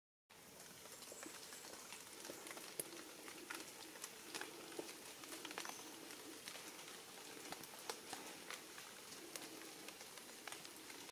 Giant Earthworm in the Santa Lucía Reserve
Giant Earthworms (family: Megascolecidae) are common in the Santa Lucía Cloud Forest Reserve. They can be up 1,5 meters long and most often observed during the rainy season; January to May.

Filmed by Holger Beck, May 2022